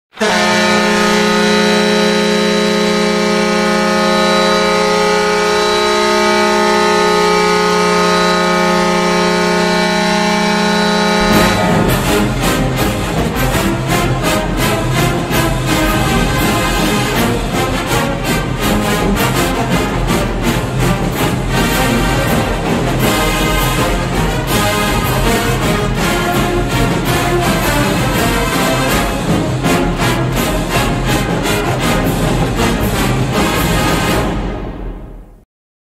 Wisconsin Badgers Goal Horn
In honor of the badgers having their best start in 22 years! We need an updated goal horn out there for the fans. Go Badgers!

-like the vid
-comment your psn username in the comments